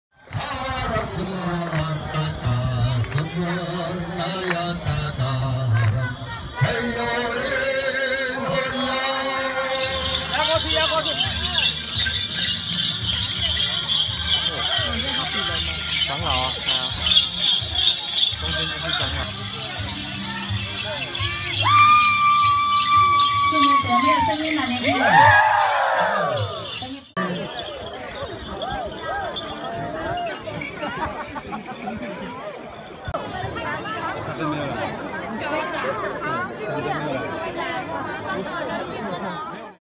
卑南族大獵祭男子凱旋歸 婦女親友花環相迎
台東縣卑南族人年祭持續，包括普悠瑪、卡大地布等參與大獵祭的勇士們，今天凱旋歸來，婦女親友們以花環相迎，熱鬧得像過年。